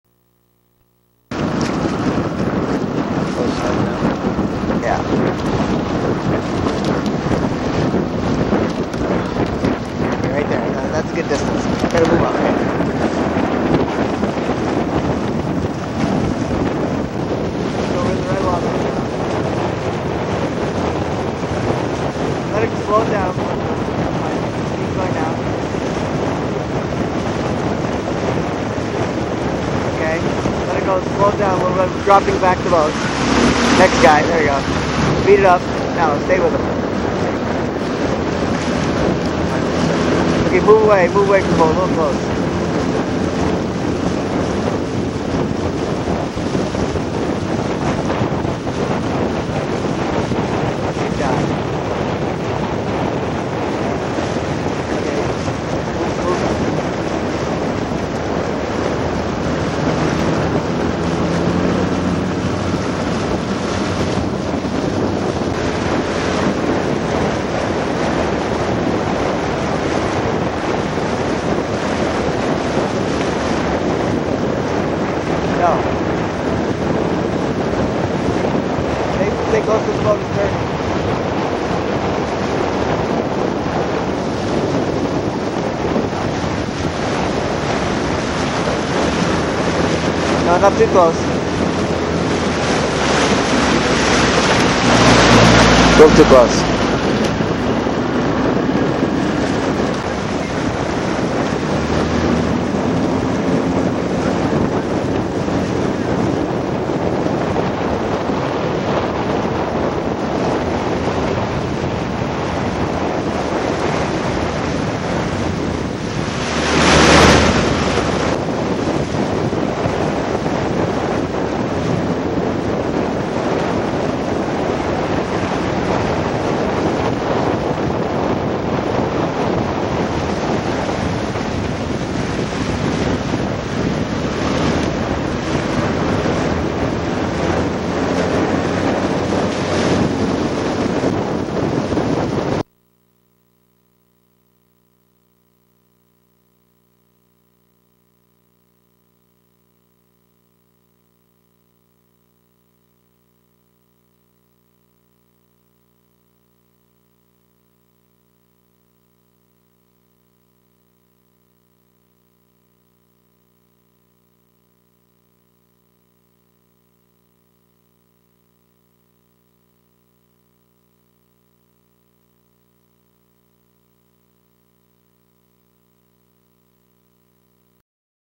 KMRC 03-01-08 Row (6 man, 6 min piece) Vid A
6 Min Pieces- 3 min, 2 min, 1 min.  Up to 2 beats each change.  Avg. 26spm.